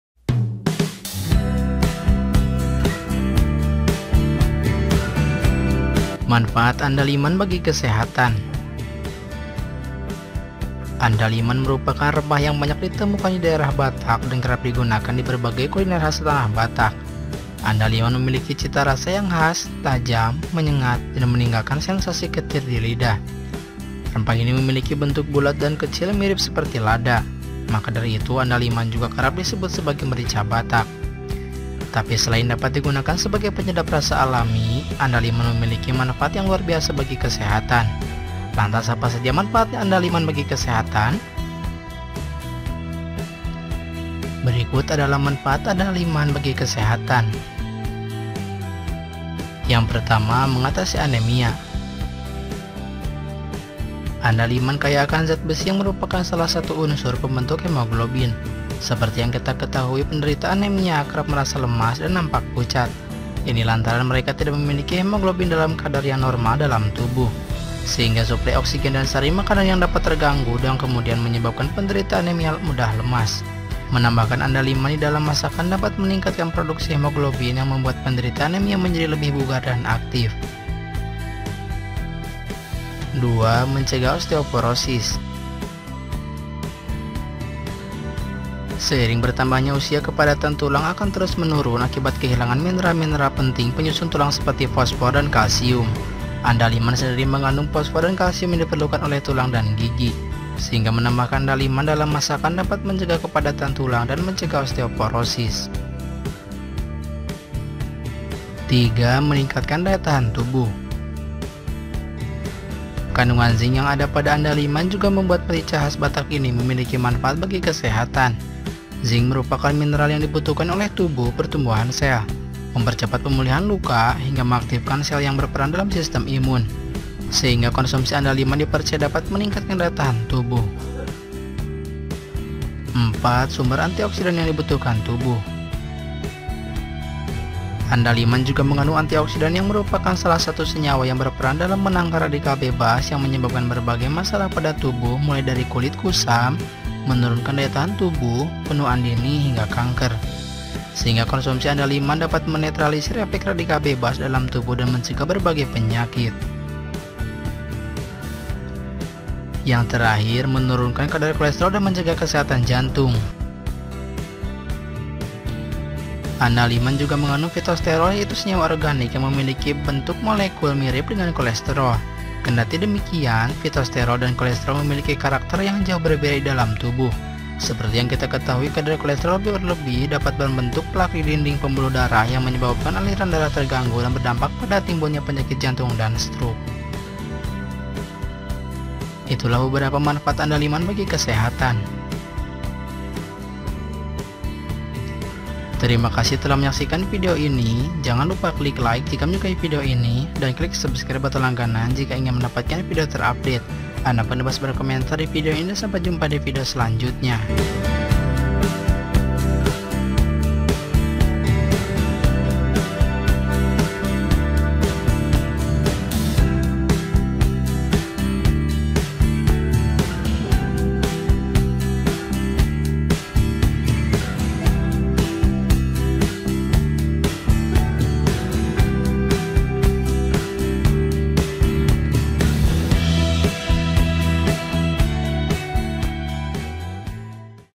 Manfaat Andaliman bagi Kesehatan
Video kali ini tentang manfaat andaliman bagi kesehatan.

Andaliman merupakan rempah yang banyak ditemukan di daerah Batak dan kerap digunakan di berbagai kuliner khas tanah Batak. Andaliman memiliki cita rasa yang khas, tajam, menyengat, dan meninggalkan sensasi ketir di lidah. Rempah ini memiliki bentuk bulat dan kecil mirip seperti lada. Maka dari itu, andaliman juga kerap disebut sebagai merica Batak. tapi selain dapat digunakan sebagai penyedap rasa alami, andaliman memiliki manfaat yang luar biasa bagi kesehatan. Lantas apa saja manfaat andaliman bagi kesehatan?

Berikut adalah manfaat andaliman bagi kesehatan.